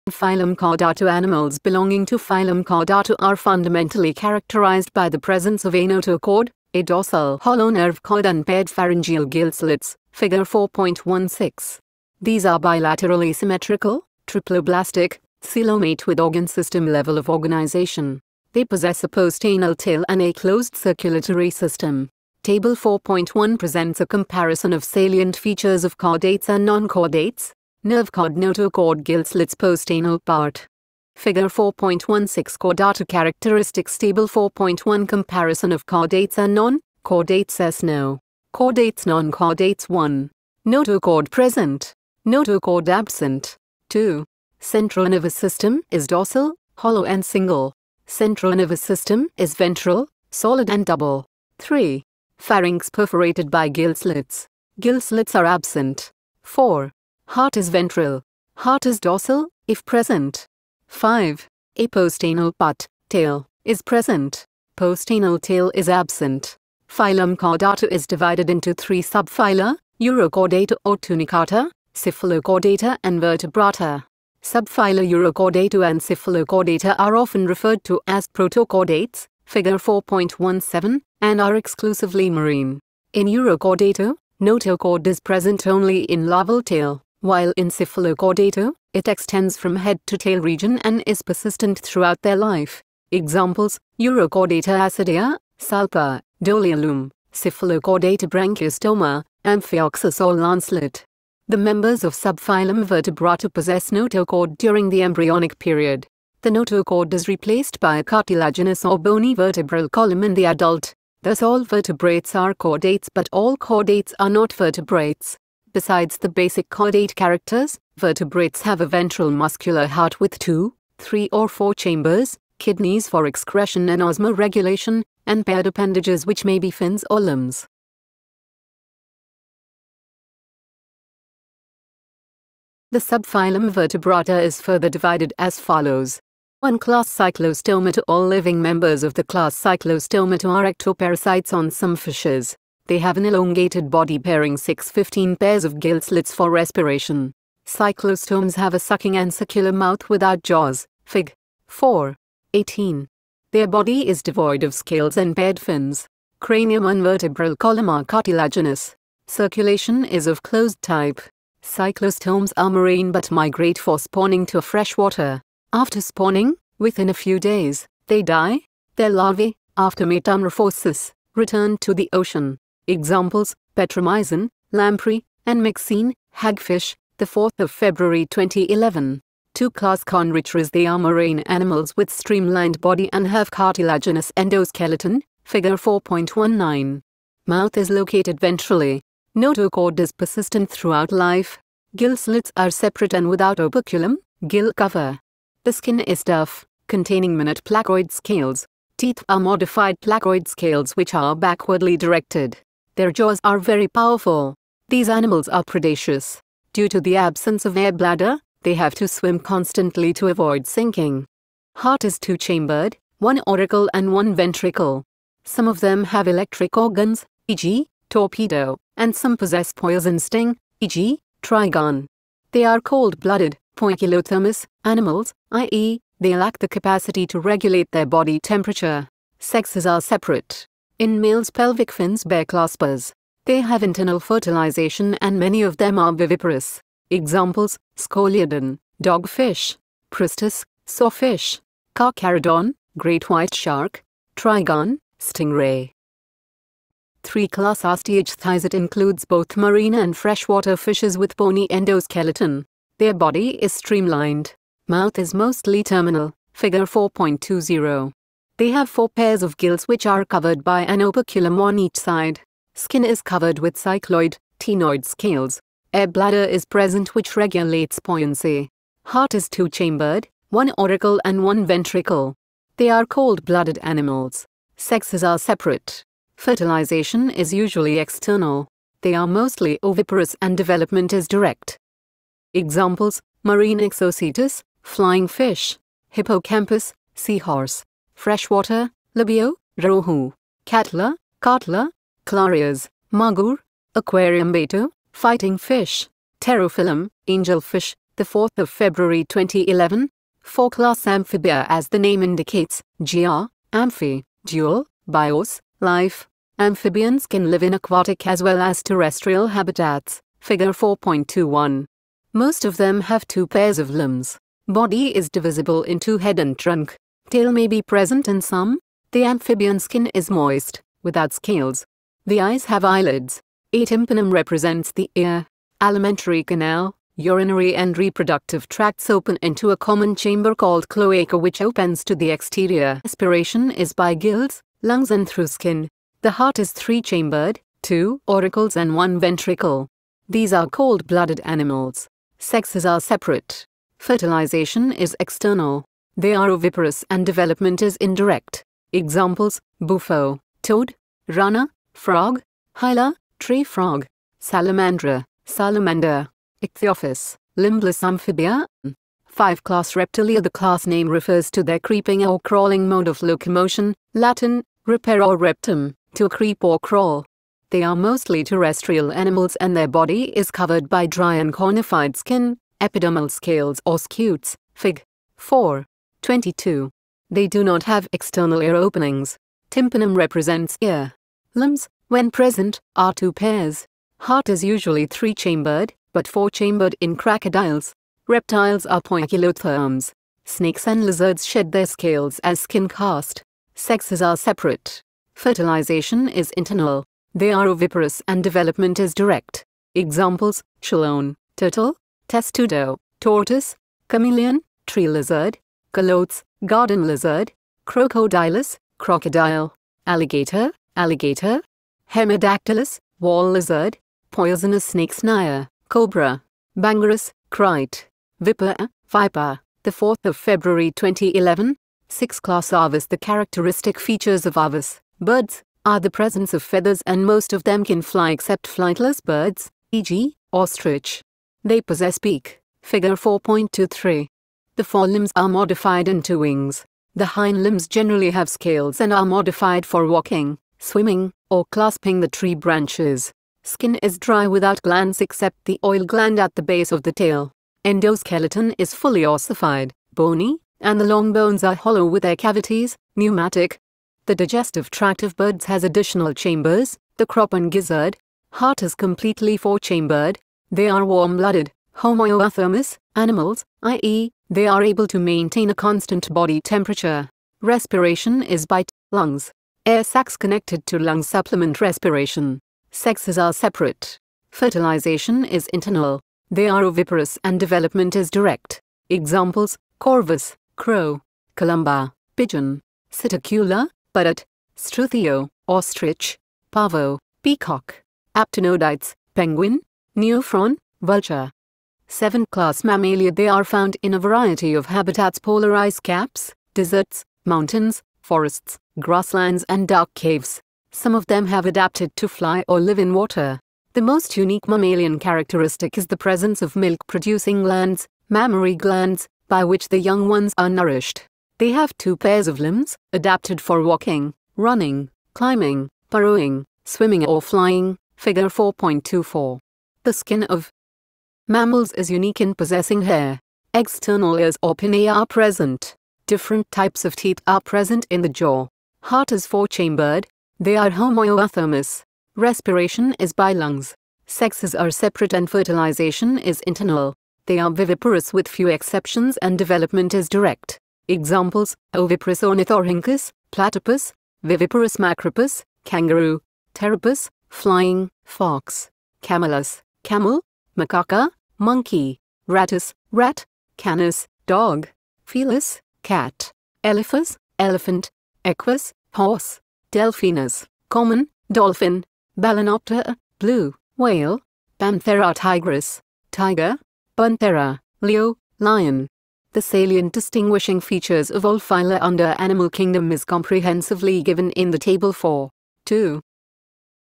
NCERT Biology Reading| Animal Kingdom Chordata NCERT Reading | Class 11 NCERT Audiobook | Class11
A small gift for all students.NCERT Class 11 Audiobook.